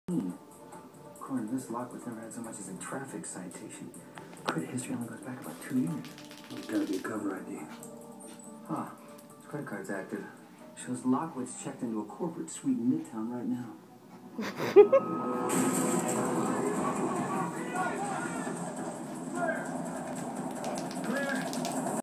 Chloee drying off
Chloee my Chihuahua drying off after a bath